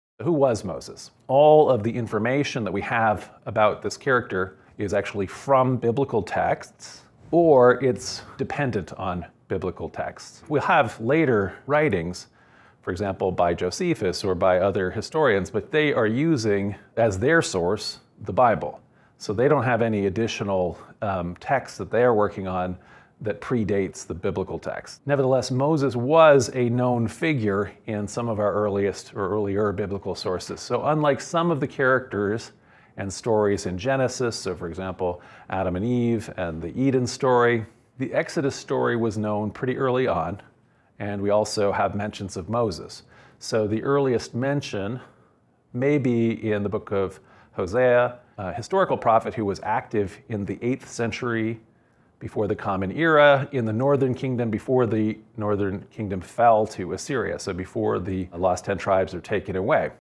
Who Was Moses: What the historical record tells us about the Exodus' hero.
From "Smashing the Ten Commandments" with John Hamer from Centre Place Toronto.

Join our live lectures every Tuesday at 7 PM EST to participate in the live discussion and ask questions to our lecturer.